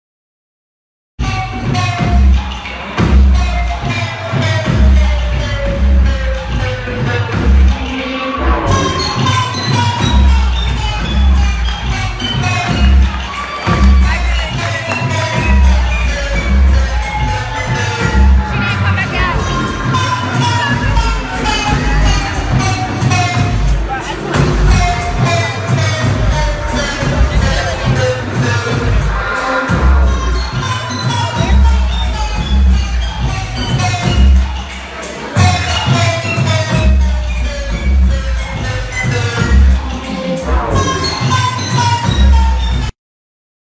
Model Falls During VFiles Show NYFW | FTV.com
Model takes an unfortunate fall on the runway during the VFILES Spring/Summer 2016 Show at New York Fashion Week